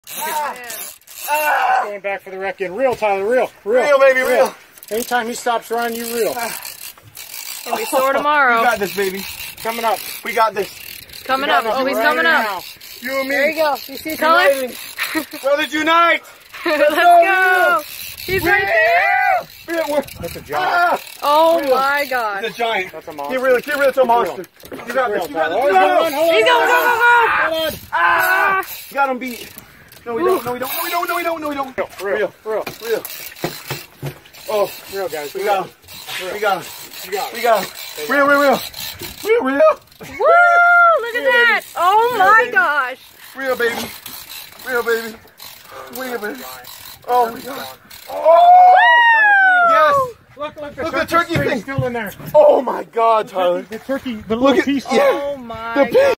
FISHING with a CHICKEN
Using chicken and turkey as giant bait for giant fish with my friend.